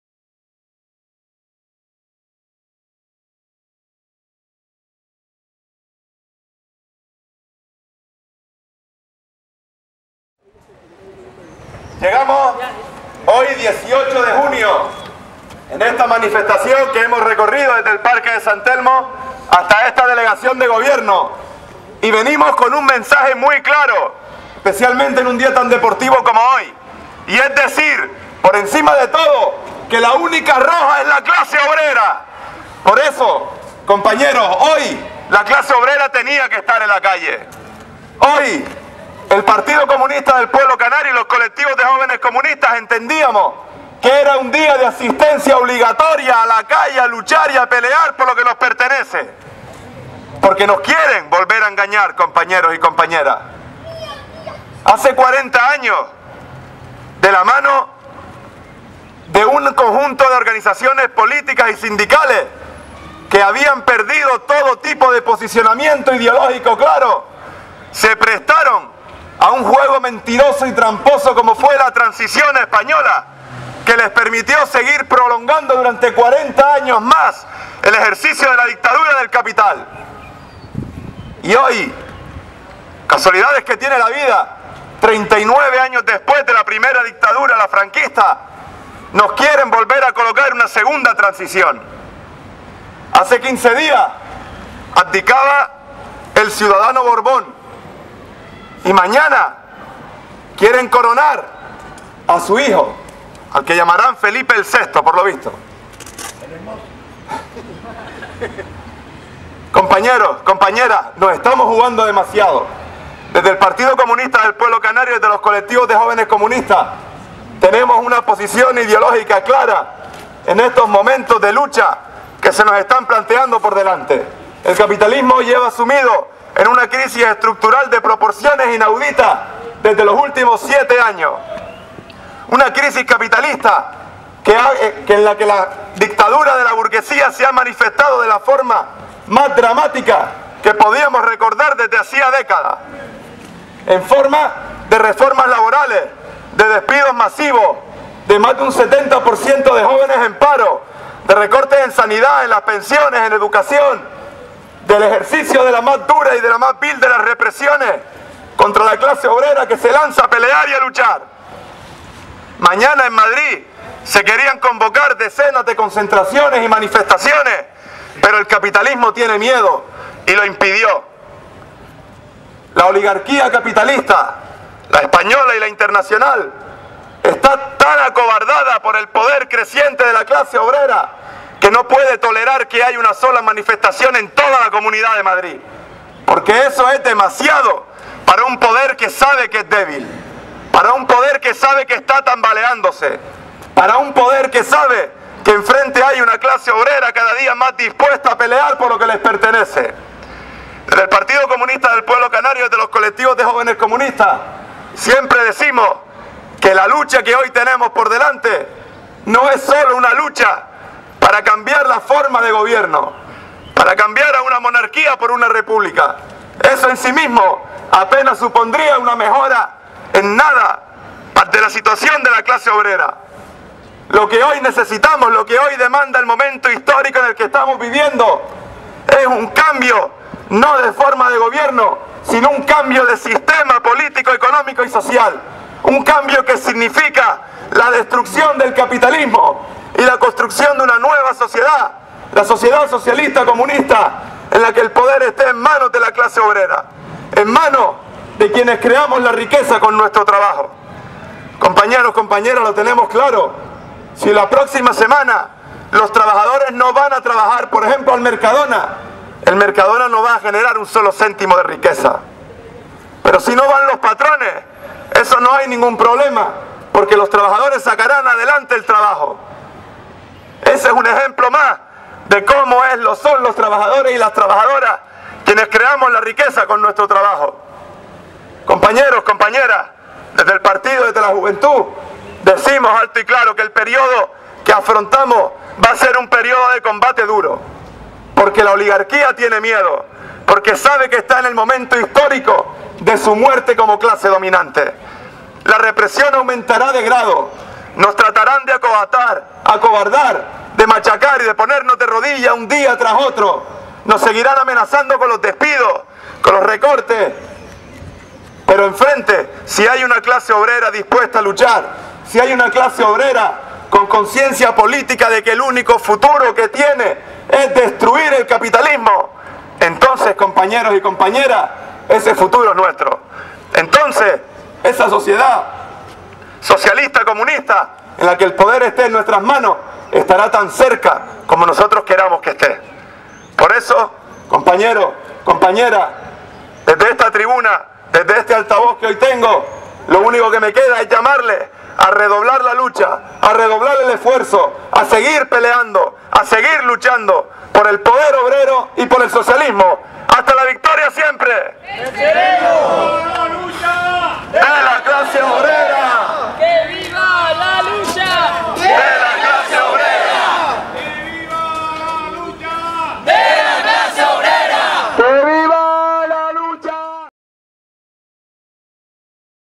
HACIA EL PODER OBRERO y la REPUBLICA SOCIALISTA 2 de 2
HACIA EL PODER OBRERO y la REPUBLICA SOCIALISTA.
Armiche Carrillo
2 de 2
PCPE-PCPC-CJC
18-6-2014

El PCPC y los CJC convocan movilizaciones en las dos principales islas ante la coronación del próximo Borbón en la jefatura de estado, para que todo siga atado y bien atado, como dijo el fascista Franco.
Ante la imposición del Borbón, ¡¡MOVILIZACIÓN POPULAR CONTINUADA Y A LA OFENSIVA!! Hay que derribar a esta monarquia que sirve de apoyo al expolio y el ataque que sufrimos por parte de los grandes monopolios y su gobierno burgués,hoy en manos del PP.
EXIJAMOS:
-Vivienda social y paralización deshaucios.
-Nacionalización de la banca y empresas públicas privatizadas.
-Seguro de paro indefinido.
-Derecho libre autodeterminación de lo pueblos.
-Eliminar: privilegios de la Iglesia, Concordato con Vaticano, ley antiterrorista,ley extranjería.
-Derogacion de contrarreforma de ley del aborto.
-Derogación de todas las contrarreformas laborales y libertad detenidos por defender derechos de la clase obrera.
-Sanidad y educacion 100% públicas.
-Salida de la OTAN, UE y €.
NO A UNA SEGUNDA TRANSICIÓN. HACIA EL PODER OBRERO y la REPUBLICA SOCIALISTA.
CONVOCATORIAS:
GRAN CANARIA: Manifestación jueves 18 a las 19:30, desde Plaza San Telmo hasta Delegación de Gobierno.
TENERIFE: Concentración miércoles 18 a las 18:00h, en Plaza Candelaria.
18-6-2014